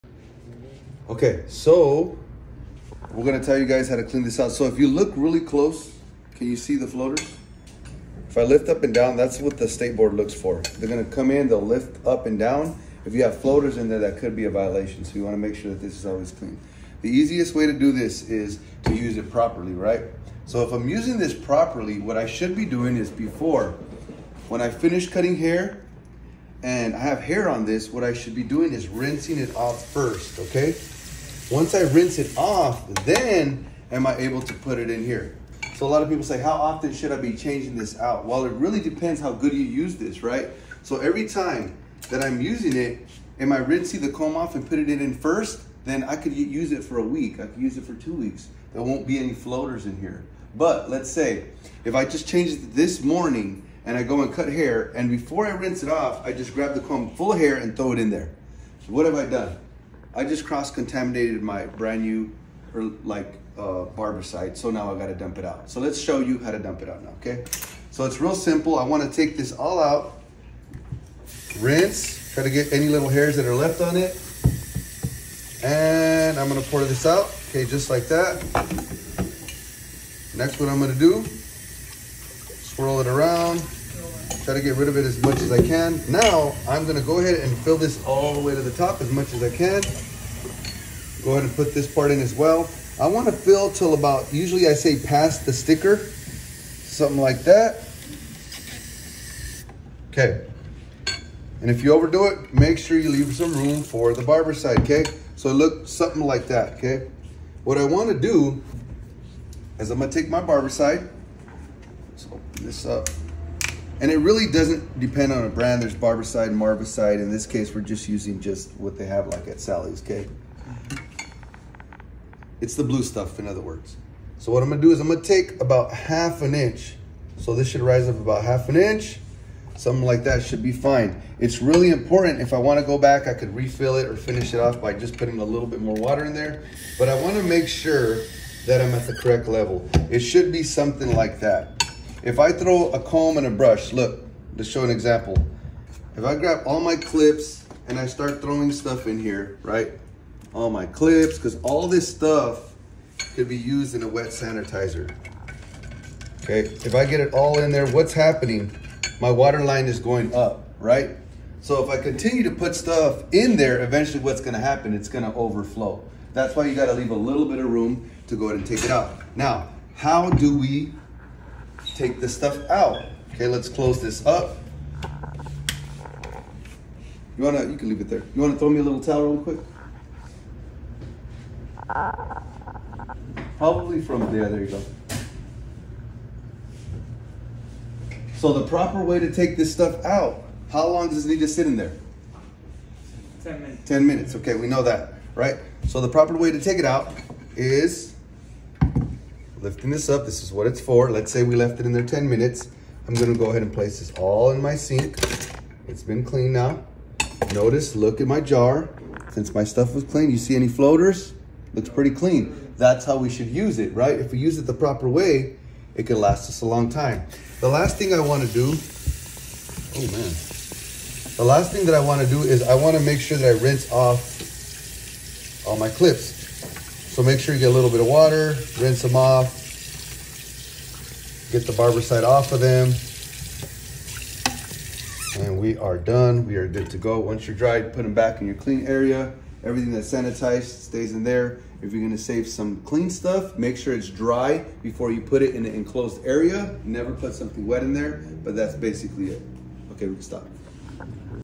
Barbers how to use a wet sanitizer (Barbicide
How to use the blue stuff (Barbicide) and how to make it last.  As a former employee for the barber board we know what the inspectors are looking for.